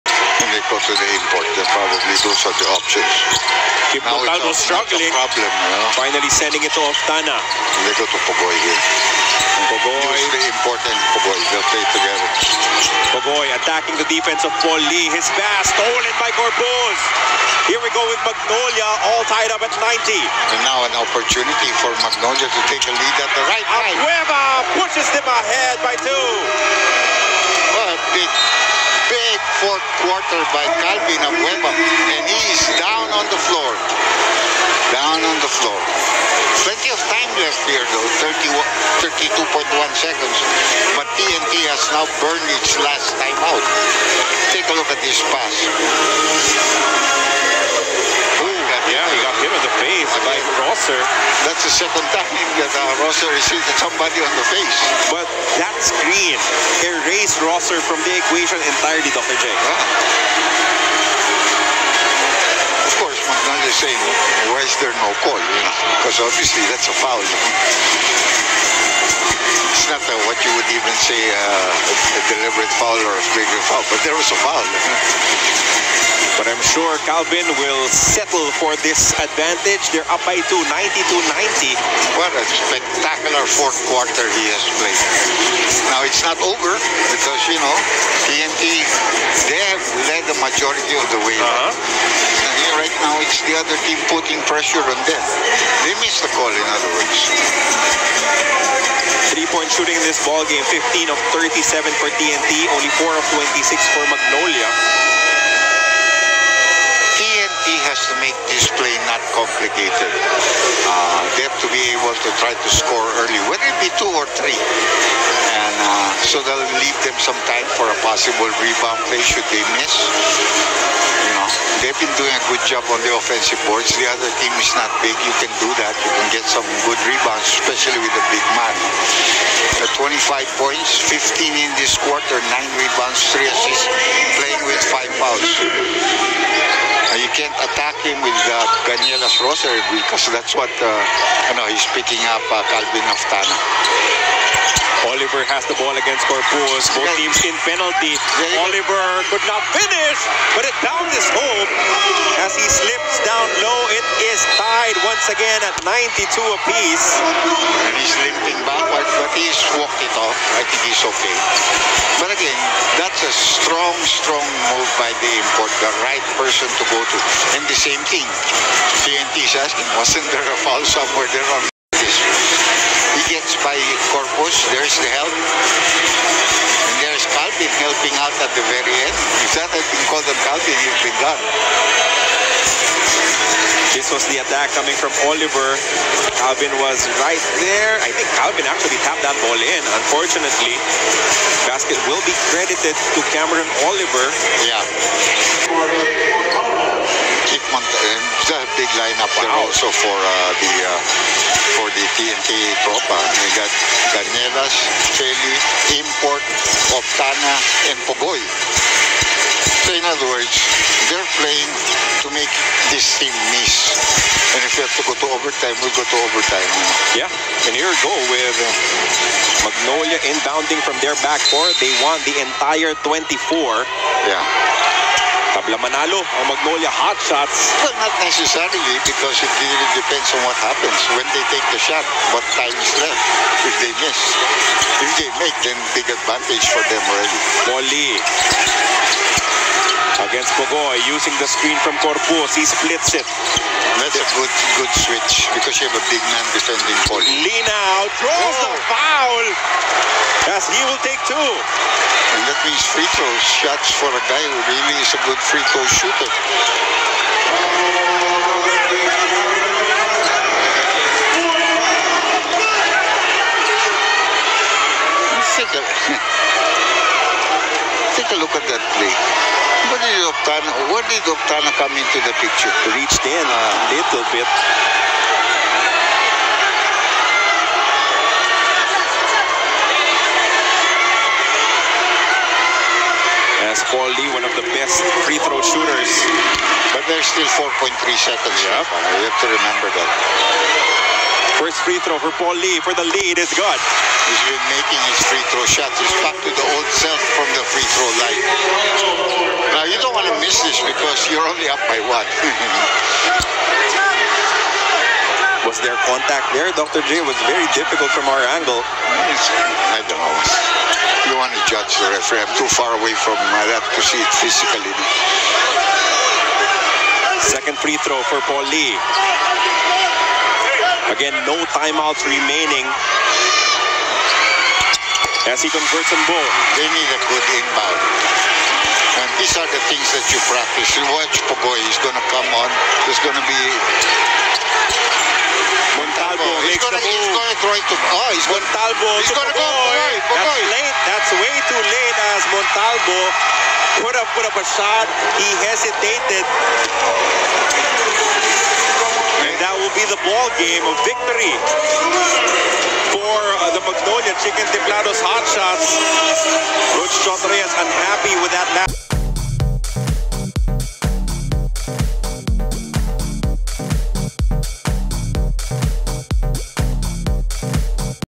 PBA TNT vs MAGNOLIA-CRUCIAL ENDING/ CHOT REYES SUMOGUD SA TABLE OPISYAL GALIT NA GALIT/10.05.22
PBA TNT vs MAGNOLIA-CRUCIAL ENDING/NANALO ANG MAGNOLIA SA SCORE NA 94-92 / COACH CHOT REYES SUMOGOD SA TABLE OPISYAL GALIT NA GALIT SA CRUCIAL PLAY NA TAWAG.

BEST PLAYER : CALVIN "THE BEAST" ABUEVA Scored: 25 point. 9 rebounds & 3 assists,.